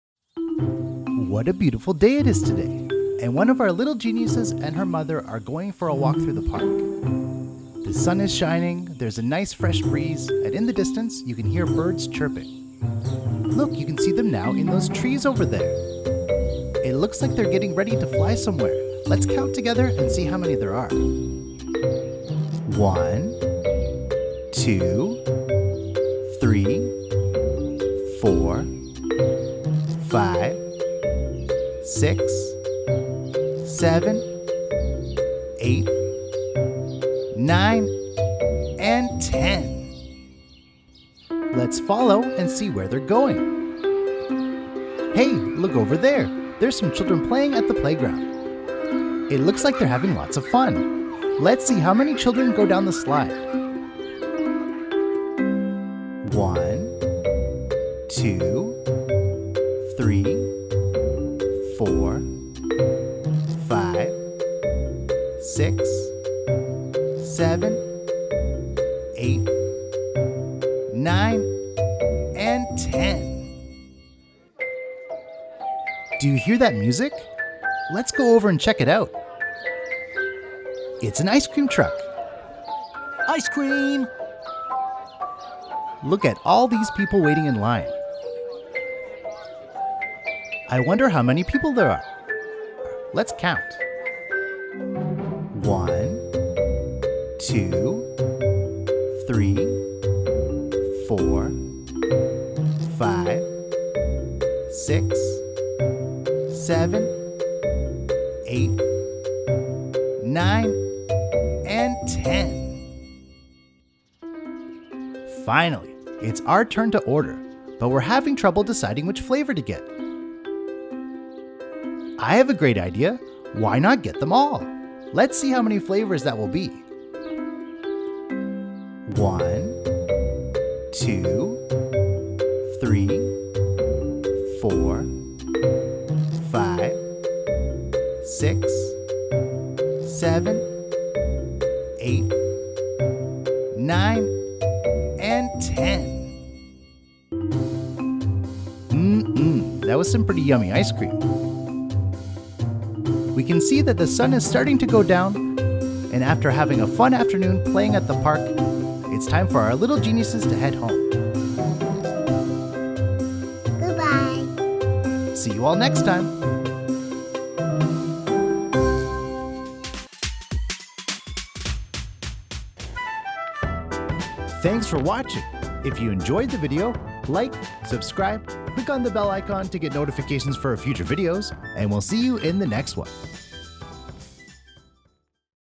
Counting Numbers TO 10 | Number Counting Story
Join us on today's adventure! Our little genius is going to have a fun afternoon at the park. Let's get ready to count! 1, 2, 3, 4, 5, 6, 7, 8, 9 and 10!

While counting and mathematics can often seem boring to children, by making counting into a story or song, it can be fun and engaging for them, which helps foster a positive attitude towards learning and education.